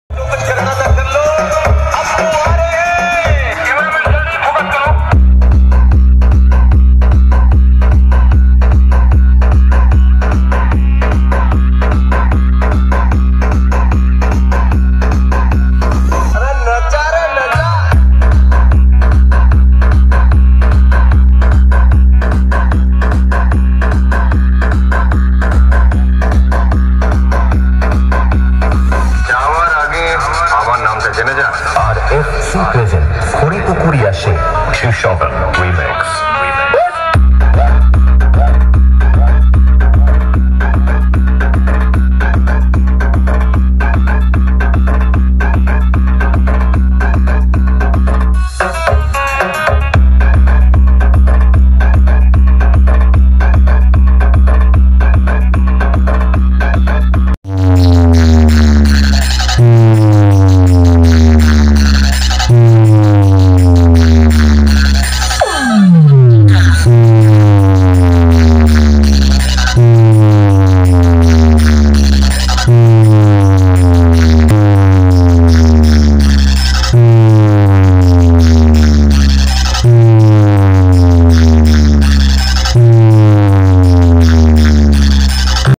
Barman Music 🔥ভদ্রকে শুরুতেই ভূমিকম্প করছে ||Odisha Box Competition
Barman music 8 Bass 9 Mid নিয়ে গনেশ পুজোর ভাসান এ ভদ্রক কাপাচ্ছে